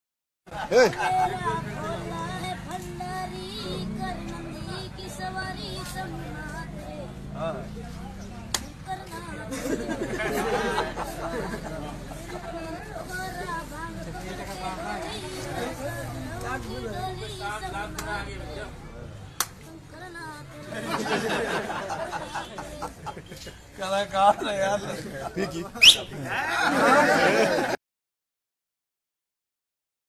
नादान बालक और पूलिस
एक छोटा बालक पुलिस वाले से ,,,,,,,,,,,,,,,मजाक मजाक मै अपनी ,,,,,,,,,,,,,,,,,,,,,,,,,,,,कलाकारी का प्रदर्शन करता हुआ